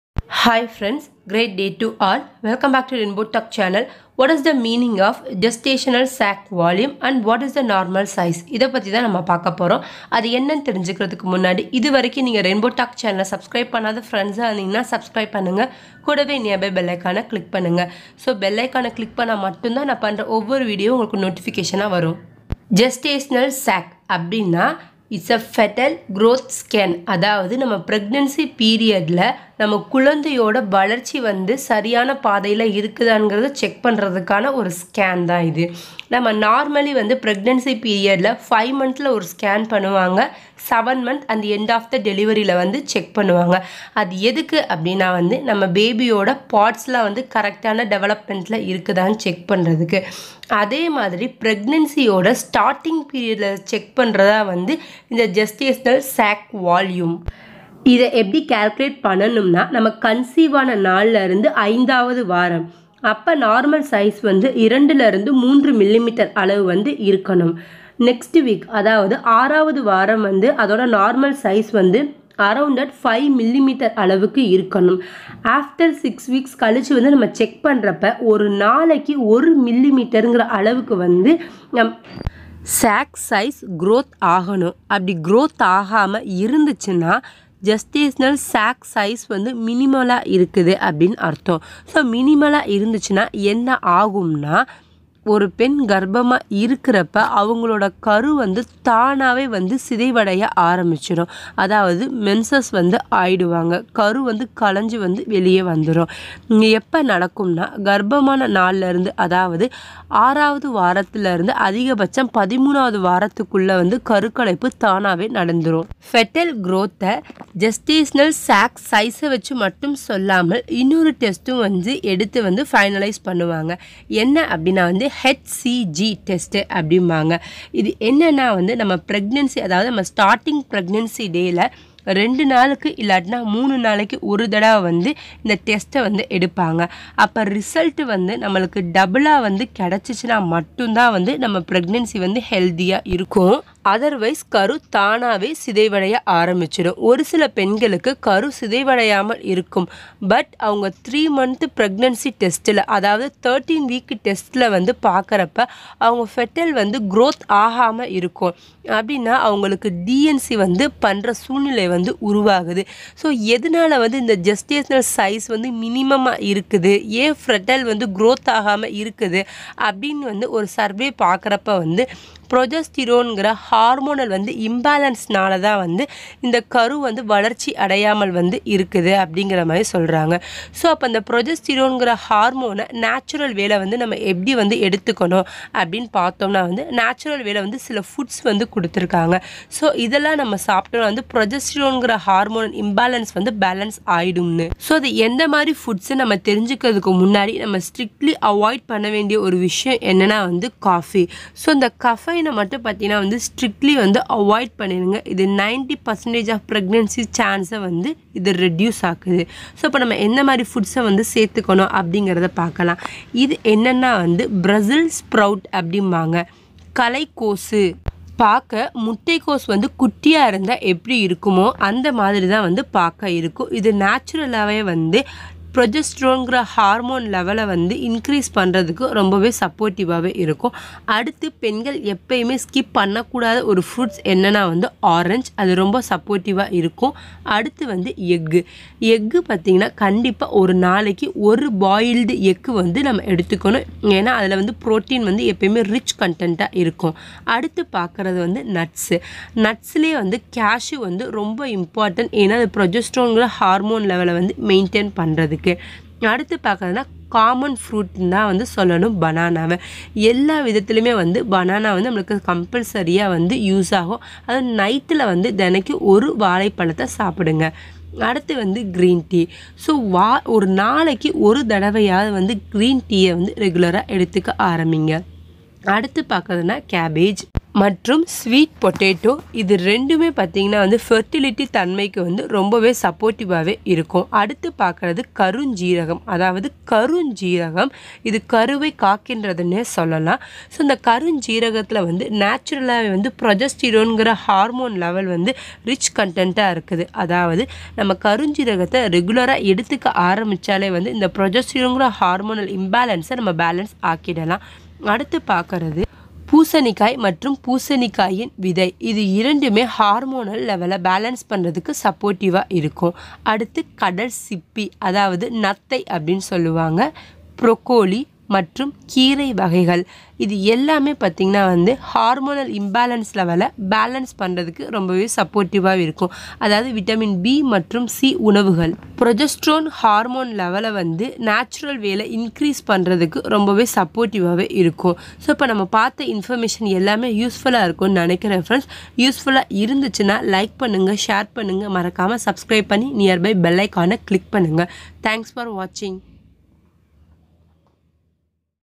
What does the meaning of gestational sac volume? and normal size in Tamil/
Here , described about  What does the meaning of gestational sac volume? meaning of gestational sac volume minimum? causes  of gestational sac volume small? what is HCG test? Gestational sac size determined by Ultrasoound test and HCG test.

உங்களுக்கு எது போன்ற வீடியோக்கள் வேண்டும் என்பதை கீழே   உள்ள comment box ல் குறிப்பிடவும் .

Rainbow Talk,...